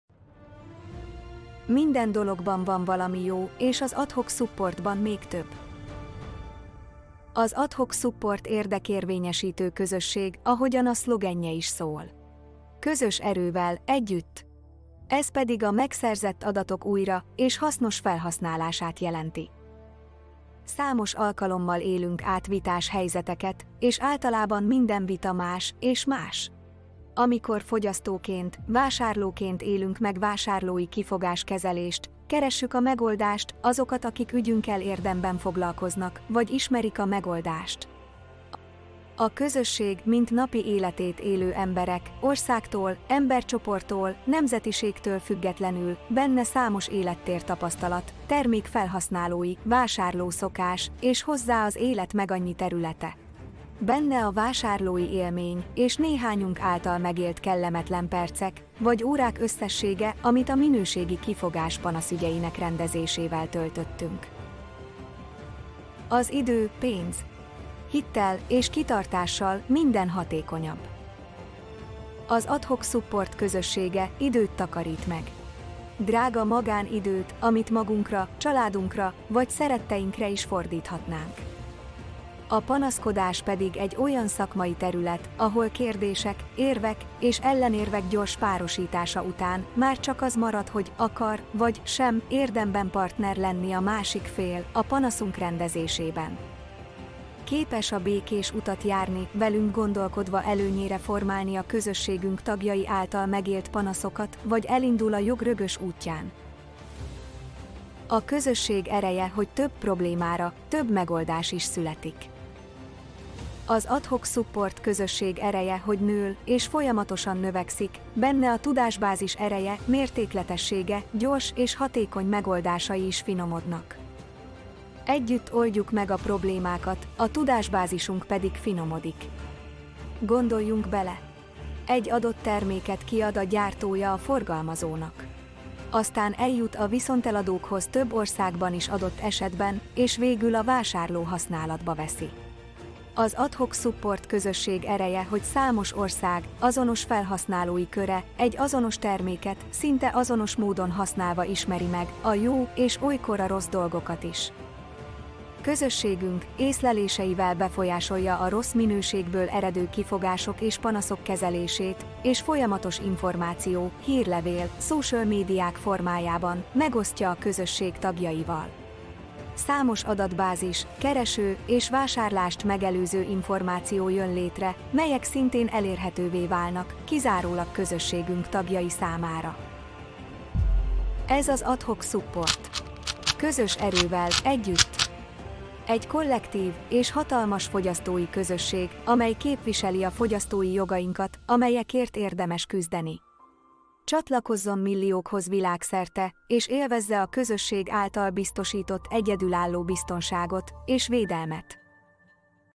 Minden dologban van valami jó és az Adhoc.Support érdekérvényesítő közösségben még több...
Minden dologban van valami jó és az Adhoc.Supportban még több!

Az Adhoc.Support érdekérvényesítő közösség, ahogyan a szlogenje is szól: Közös erővel, együtt! - ez pedig a megszerzett adatok, újra és hasznos felhasználását jelenti.

Számos alkalommal élünk át vitás helyzeteket és általában minden vita más és más.

Amikor fogyasztóként, vásárlóként élünk meg vásárlói kifogás kezelést, keressük a megoldást, azokat akik ügyünkkel érdemben foglalkoznak vagy ismerik a megoldást.

A közösség, mint napi életét élő emberek, országtól, embercsoporttól, nemzetiségtől függetlenül: benne számos élettér tapasztalat, termék felhasználói, vásárló szokás és hozzá az élet megannyi területe.

Benne a vásárlói élmény és néhányunk által megélt kellemetlen percek vagy órák összessége, amit a minőségi kifogás panaszügyeinek rendezésével töltöttünk.

Az idő pénz: hittel és kitartással minden hatékonyabb!

Az Adhoc.Support közössége időt takarít meg.

Drága magán időt, amit magunkra, családunkra vagy szeretteinkre is fordíthatnánk.

A panaszkodás pedig egy olyan szakmai terület, ahol kérdések, érvek és ellenérvek gyors párosítása után már csak az marad: akar vagy sem érdemben partner lenni a másik a panaszunk rendezésében.

Képes a békés utat járni, velünk gondolkodva előnyére formálni a közösségünk tagjai által megélt panaszokat, vagy elindul a jog rögös útján?

A közösség ereje, hogy több problémára több megoldás is születik.

Az Adhoc.Support közösség ereje, hogy nől és folyamatosan növekszik, benne a tudásbázis ereje, mértékletessége, gyors és hatékony megoldásai is finomodnak.

Együtt oldjuk meg a problémákat, a tudásbázisunk pedig finomodik.
Gondoljunk bele: egy adott terméket kiad a gyártója a forgalmazónak. Aztán eljut a viszonteladókhoz, több országban is adott esetben és végül a vásárló használatba veszi.

Az Adhoc.Support közösség ereje, hogy számos ország, azonos felhasználói köre, egy azonos terméket, szinte azonos módon használva ismeri meg a jó és olykor a rossz dolgokat is.

Közösségünk észleléseivel befolyásolja a rossz minőségből eredő kifogások és panaszok kezelését és folyamatos információ (hírlevél, social médiák) formájában megosztja a közösség tagjaival.

Számos adatbázis, kereső és vásárlást megelőző információ jön létre, melyek szintén elérhetővé válnak kizárólag közösségünk tagjai számára.

Ez az Adhoc.Support! Közös erővel, együtt!

Egy kollektív és hatalmas fogyasztói közösség, amely képviseli a fogyasztói jogainkat, amelyekért érdemes küzdeni!

Csatlakozzon milliókhoz világszerte, és élvezze a közösség által biztosított egyedülálló biztonságot és védelmet.